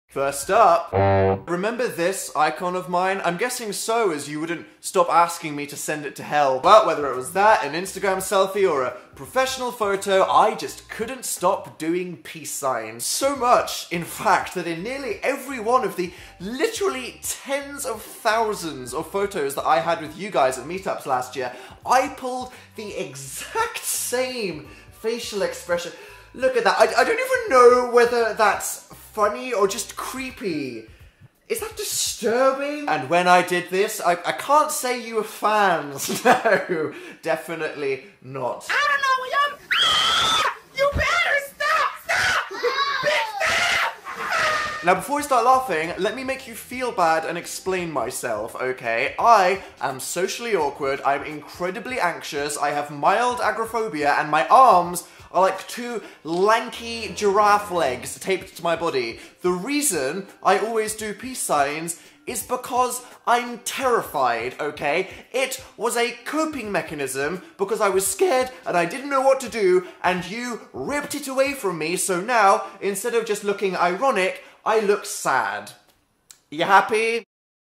Peace signs & mild agoraphobia
Dan discusses how he uses peace signs as a coping mechanism.

Transcript:

First up: the peace signs. Remember this icon of mine? I’m guessing so, as you wouldn’t stop asking me to send it to hell. Well, whether it was that, an Instagram selfie, or a professional photo, I just couldn’t stop doing peace signs. So much, in fact, that in nearly every one of the literally tens of thousands of photos that I had with you guys at meet-ups last year, I pulled the exact same facial expression… look at that. I don’t even know whether that’s funny or just creepy. Is that disturbing? And when I did this, I— I can’t say you were fans. No. Definitely not.
Now before you start laughing, let me make you feel bad and explain myself, okay? I am socially awkward, I’m incredibly anxious, I have mild agoraphobia, and my arms are like two lanky giraffe legs taped to my body. The reason I always do peace signs is because I’m terrified, okay? It was a coping mechanism because I was scared and I didn’t know what to do, and you ripped it away from me, so now, instead of just looking ironic, I look sad. You happy?